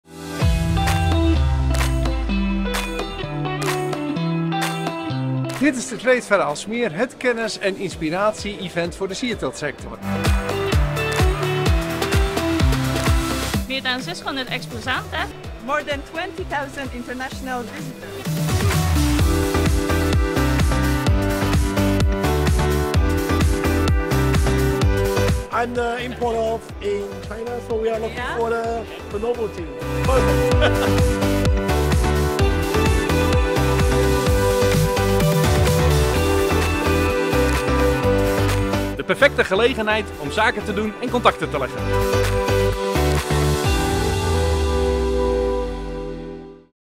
Trade Fair Aalsmeer 2023
We kijken terug op een zeer geslaagde editie van Trade Fair Aalsmeer op 7, 8 en 9 november. Het terrein van Royal FloraHolland was drie dagen lang het epicentrum van de sierteeltsector en trok maar liefst 21.000 bezoekers van over de hele wereld.

We can look back on a very successful edition of Trade Fair Aalsmeer on 7, 8 and 9 November. For three days, the Royal FloraHolland grounds were the epicentre of the floriculture sector, attracting no less than 21,000 visitors from all over the world.